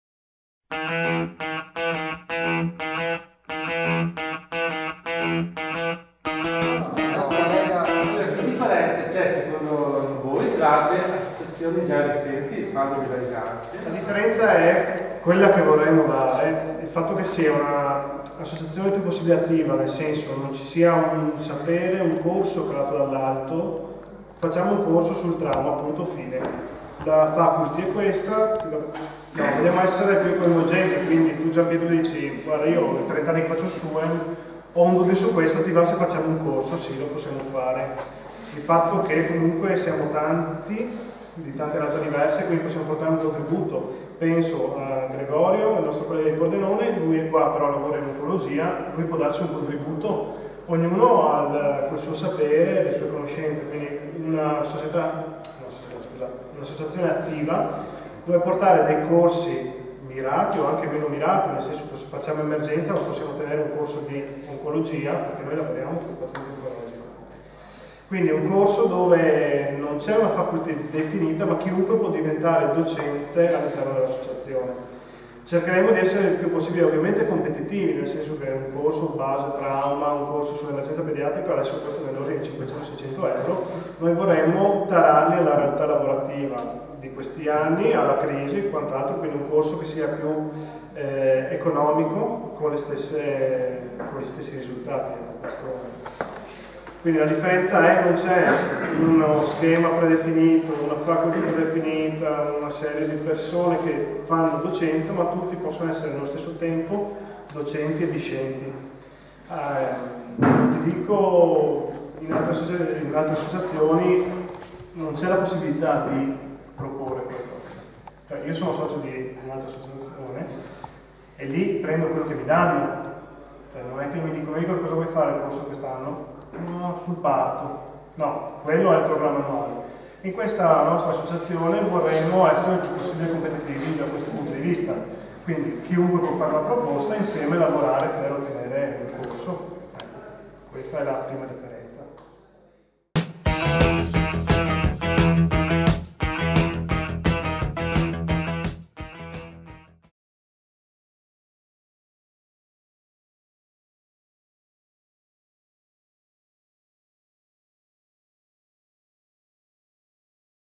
Cosa vi differenzia da altre associazioni? (Presentazione I.E.T. 5 (26/9/13): Domanda 2)
La presentazione ufficale di Infermieri Emergenza Territoriale in alcuni brevi video. Il quarto è la domanda del pubblico: "che cosa vi differenzia dalle altre associazioni?" risponde Igor Sist. Il gruppo "Infermieri Pordenone" dopo averli intervistati è andato ad ascoltare armato di telecamere e tutto il necessario: ecco il risultato.

Riprese: Segatto Gregorio
Montaggio post produzione: Max Fassetta

Per saperne di più:
- il post su infermieri pordenone: